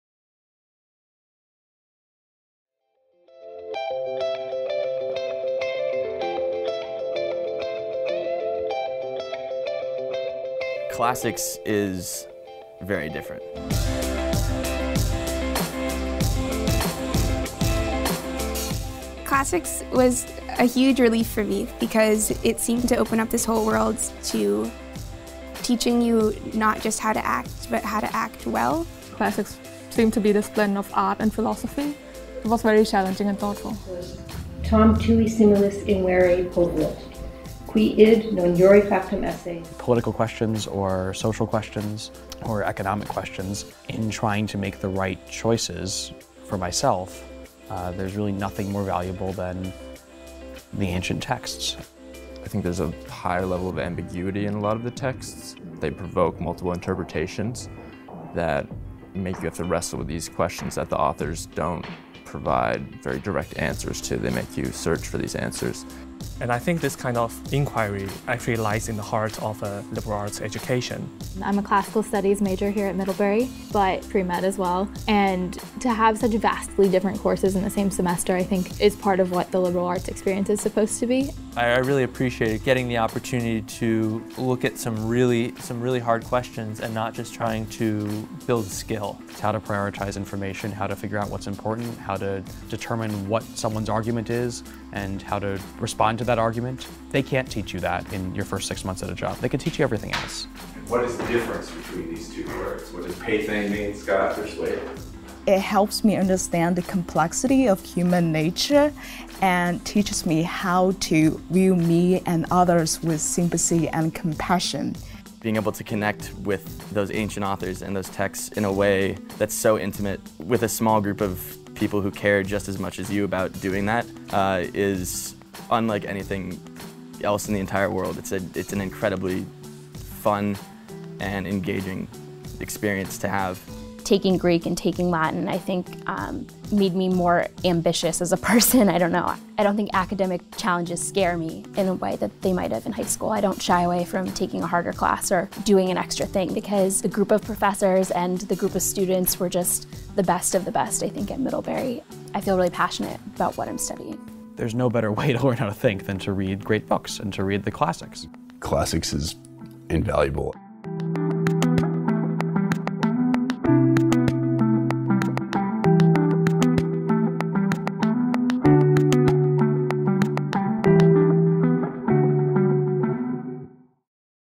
Middlebury - 2016 - The Classics at Middlebury
There is more to the studying the classics than one might expect. In this short video, we hear from students how immersion in classical texts and languages enriches one's personal and intellectual life at Middlebury.